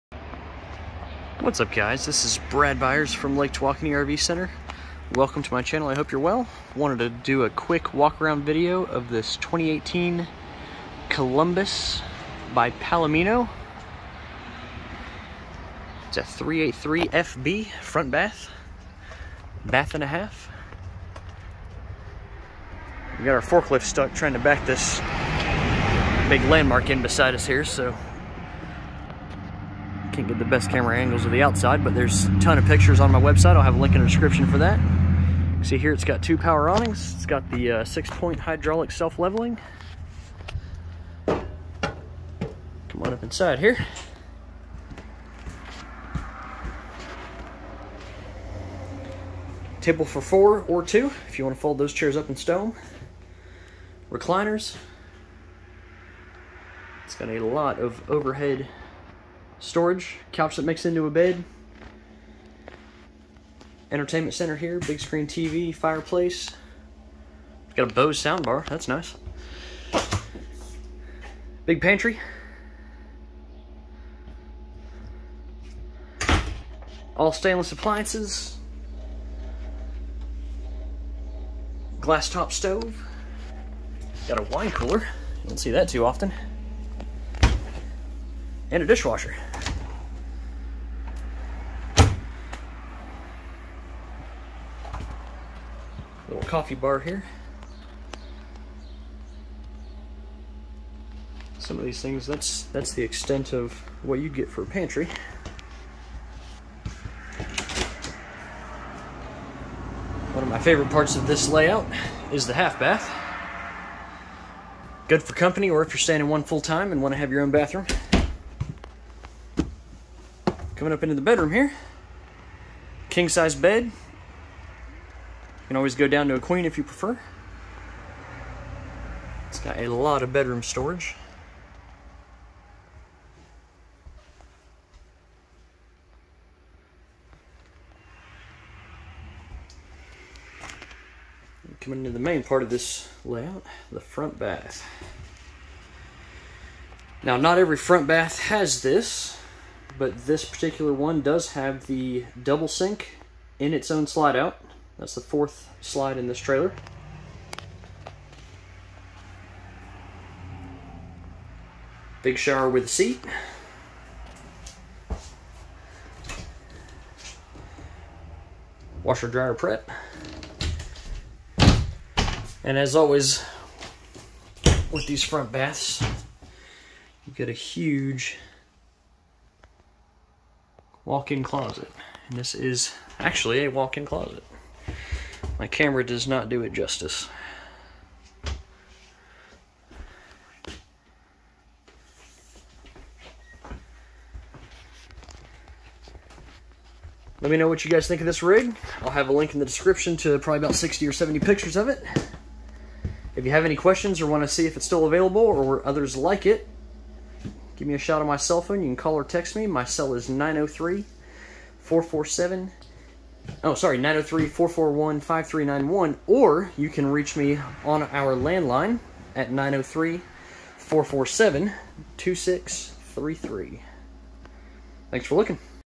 (SOLD) HUGE Front Bath W/ Downstairs Half Bath! Palomino Columbus 1492 383FB 4 Slide Fifth Wheel!
New Arrival!
2018 Forest River Palomino Columbus 383FB 1492 Edition 4 Slide Rear Living Fifth Wheel!